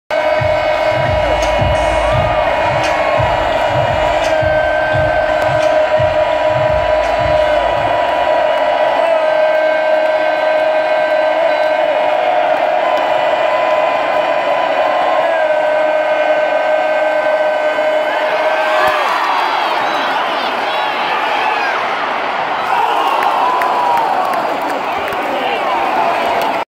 Alabama vs Texas A&M 10/07/17 the 12 man
the 12 man was loud against Bama .